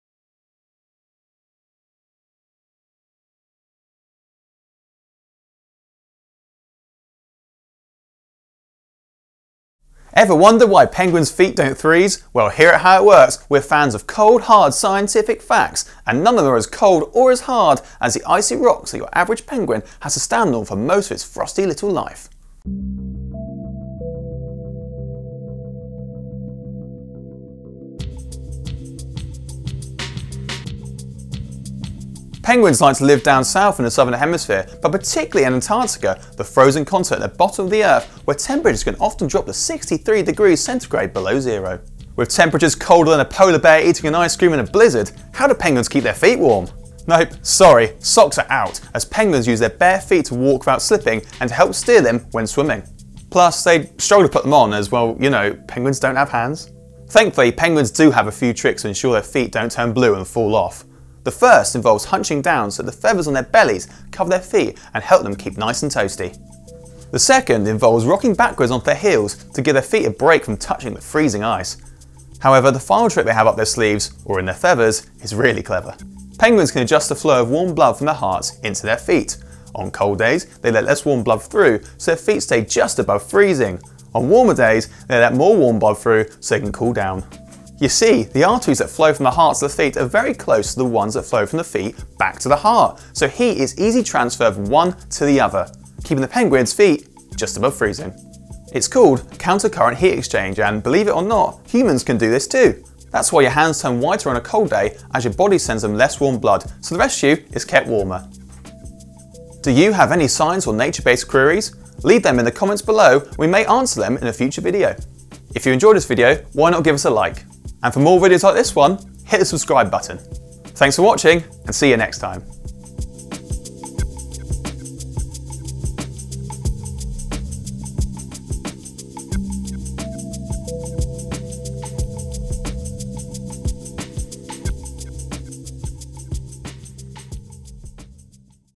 Why don’t Penguins’ feet freeze?
Ever wondered why penguins’ feet don’t freeze? Well here at How It Works, we’re fans of cold, hard scientific facts and none of them are as cold or as hard as the icy rocks that your average penguin has to stand on for most of its frosty little life.

This video is brought to you in association with Cubs 100.

Or purchase the latest issue of the magazine from the Imagine Shop